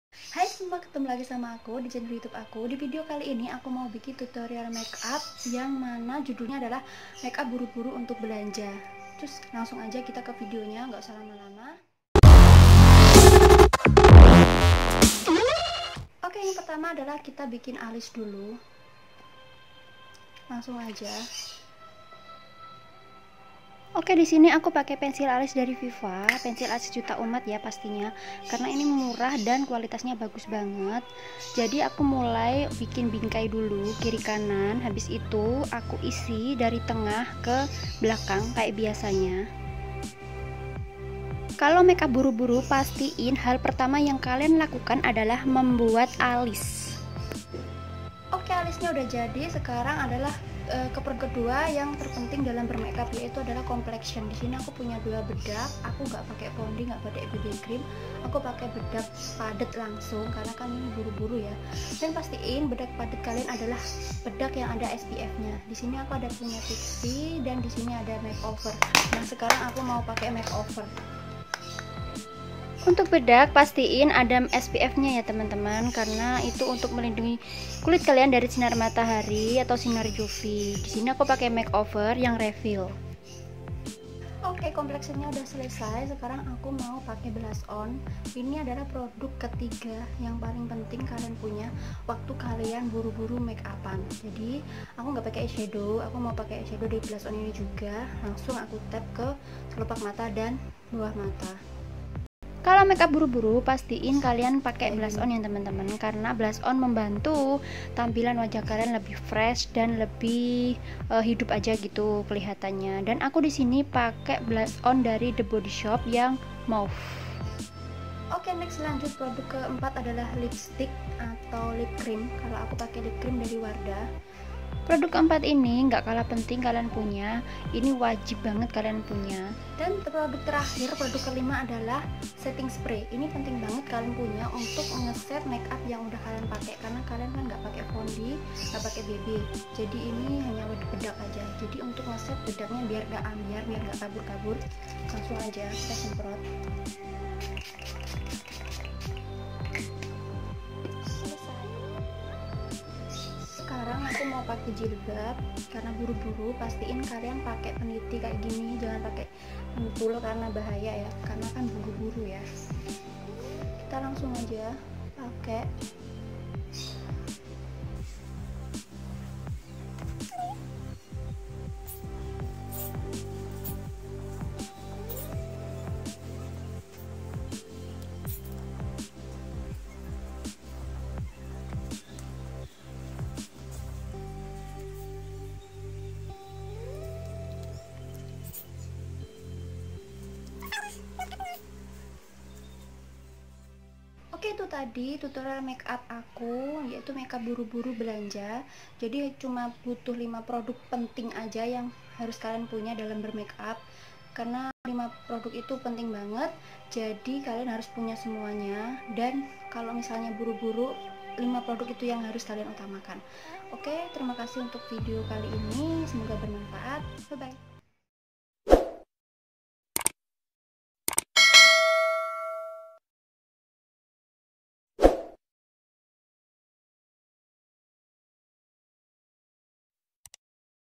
Tutorial Make Up Buru-buru Mau Belanja|| Pakai 5 Produk Saja
Produk:
1. Viva Pensil Alis Warna Brown
2. Make Over  Perfect Cover Two Way Cake 03 Maple
3. The Body Shop Blush On 02 Guava
4. Wardah Lipcream 03 See You Late
5. Pixy Setting Spray